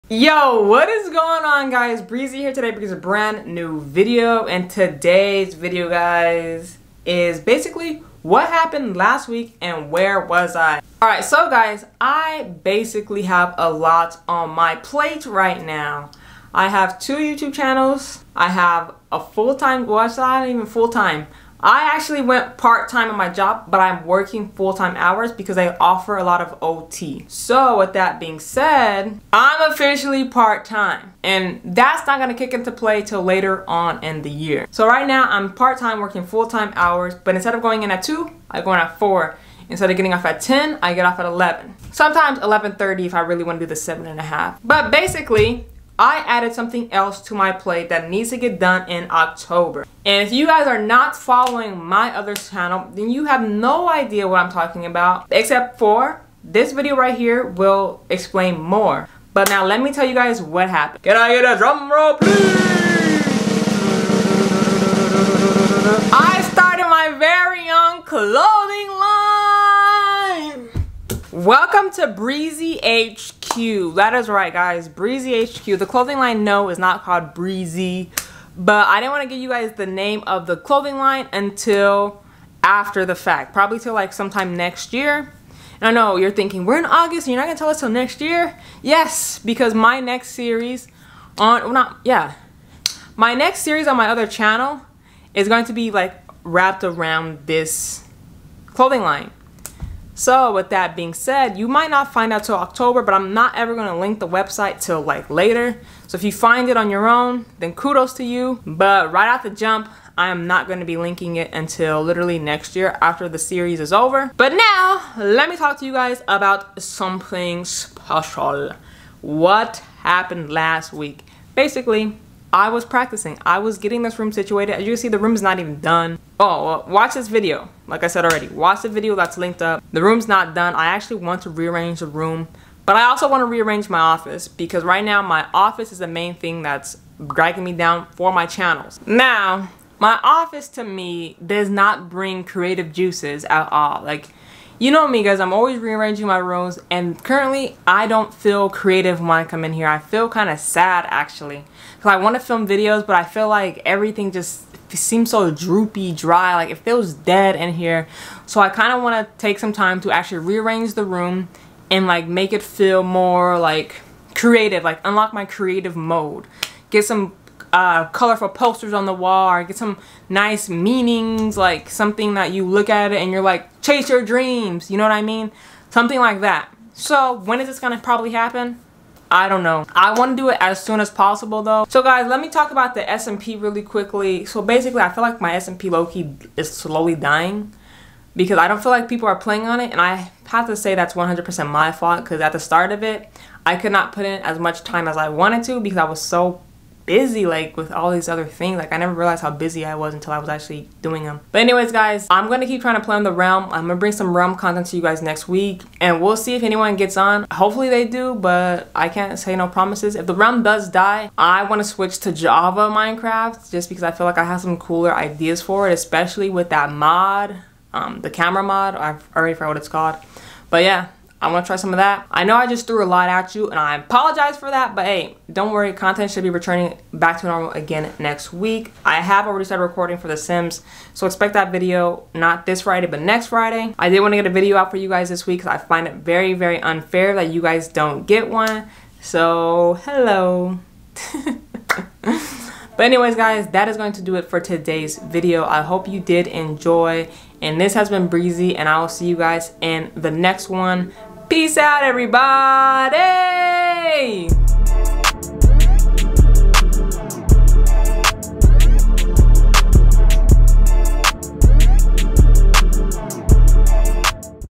Something big is coming...
Enjoyed this video? Watch the latest video here - Isiah's Quest To Restore His Family | The Sims 4
7 Views - Mar 16, 2024

Sorry about no videos last week guys but something in my life is going crazy! As stated in the video I am starting my own clothing line/brand that series is going to be taking place on /MoreBreezy - Check out the channel if you're interested.